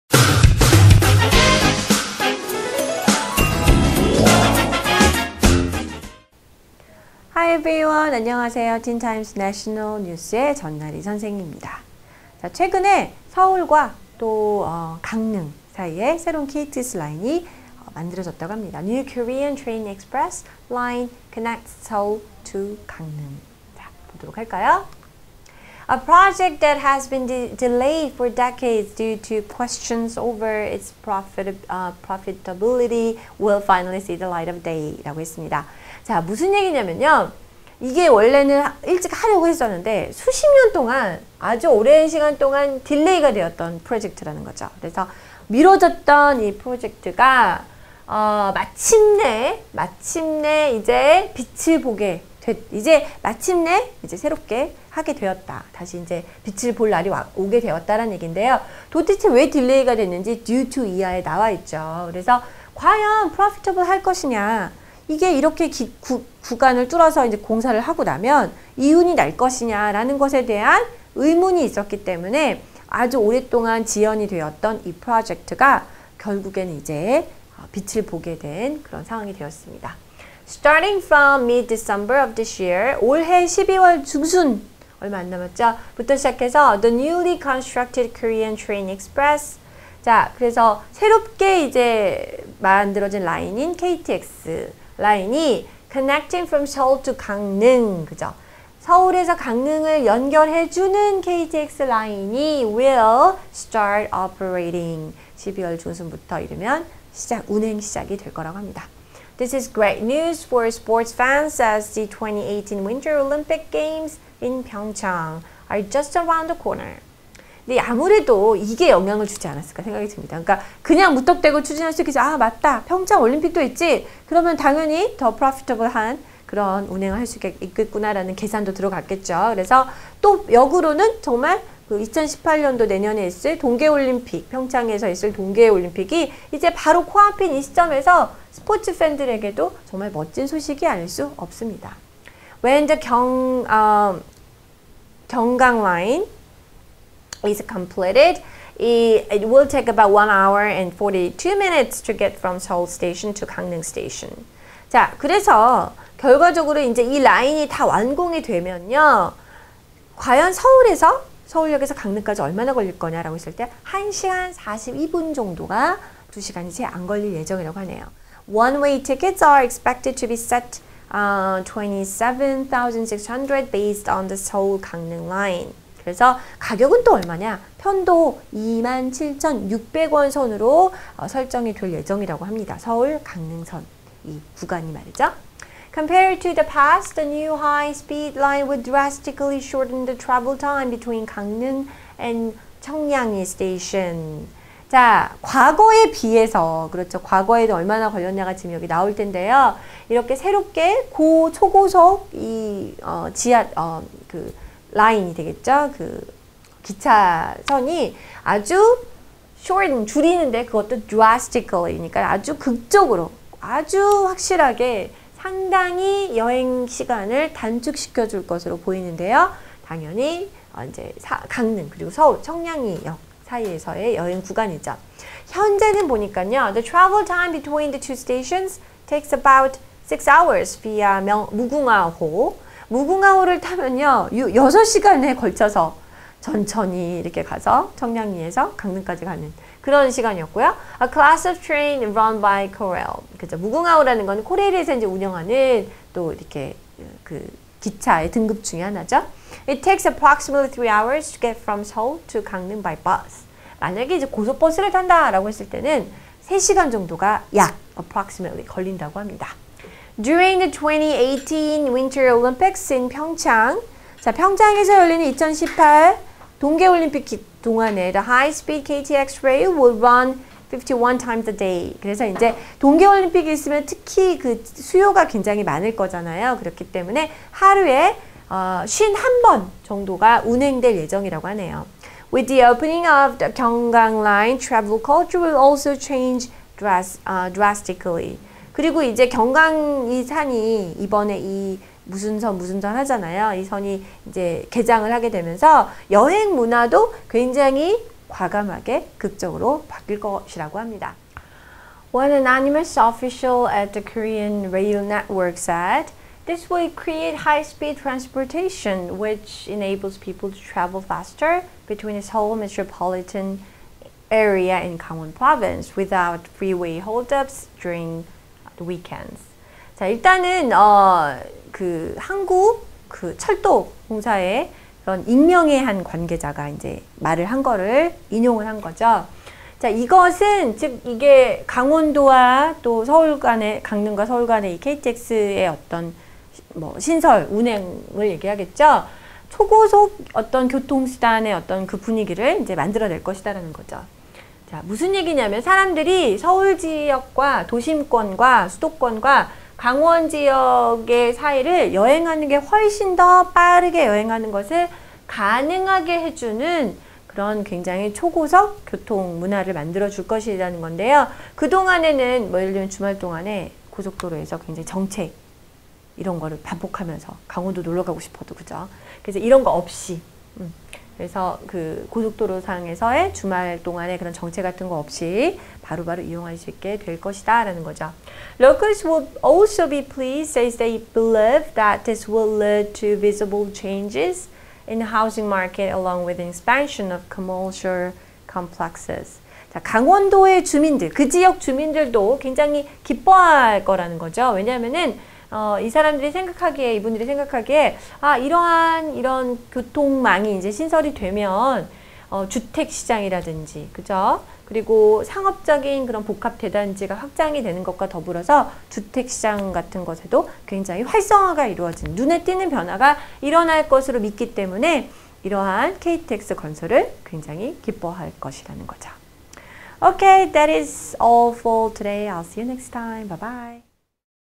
New Korean Train Express... - 전나리 선생님과 함께 하는 National News I 동영상 강의 ( The Teen Times - No.857 )
청소년 영자신문 틴타임즈 857호 National News I 동영상 강의

신설 KTX 시대, 서울과 강릉 연결